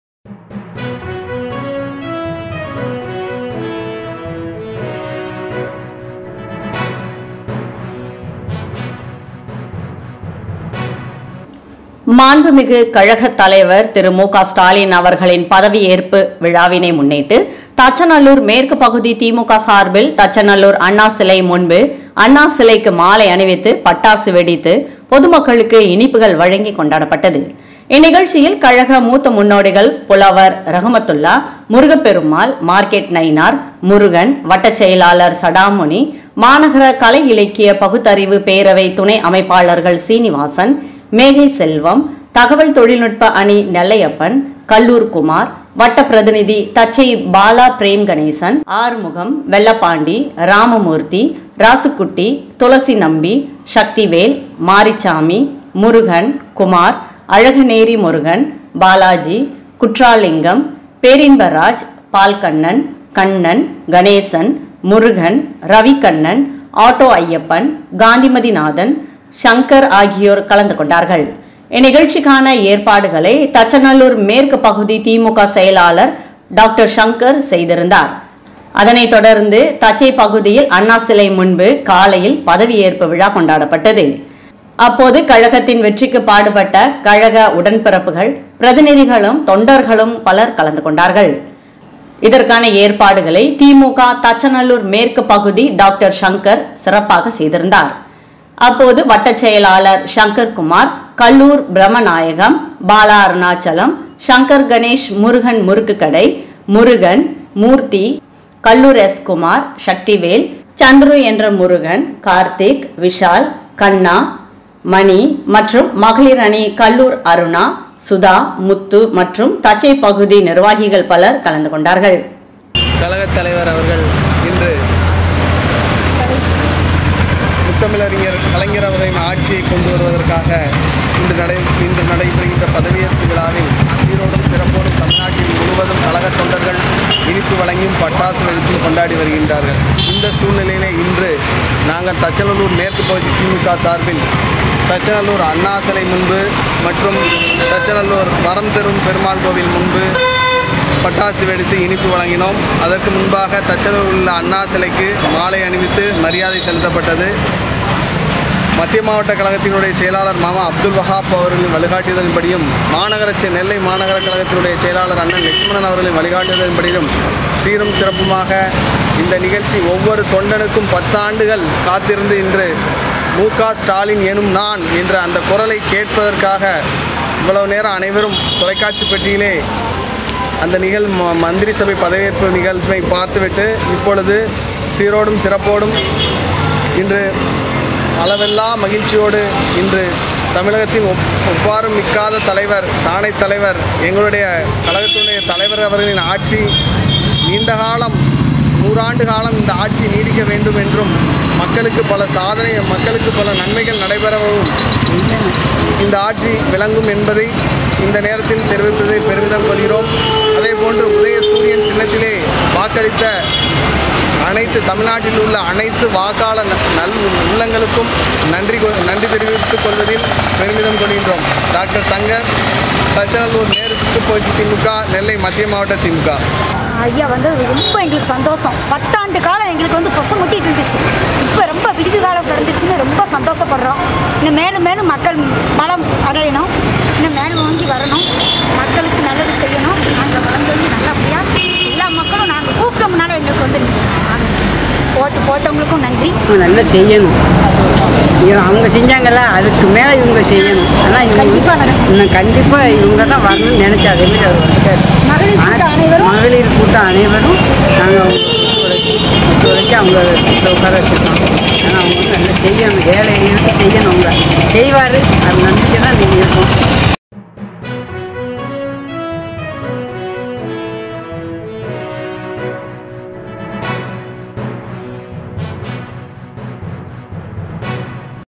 தச்சநல்லூர் மேற்கு பகுதி தி.மு.க வினர் பட்டாசு வெடித்து கொண்டாட்டம்
கல்லூர் எம். பிரமநாயகம், எஸ்.பாலா அருணாச்சலம்.
எஸ். சங்கர் கணேஷ் முருகன் முருக்கு கடை.
 ஏ முருகன்
 எம் மூர்த்தி
கல்லூர் எஸ். குமார்
 ஈ. சக்திவேல்
எஸ் சந்துரு என்று முருகன். கார்த்திக். விஷால் .ஆ.கண்ணா
 என்எஸ்மணி . மற்றும் மகளிர் அணி கல்லூர் அருணா. சுதா. முத்து.
 மற்றும்தச்சைப்பகுதி திமுக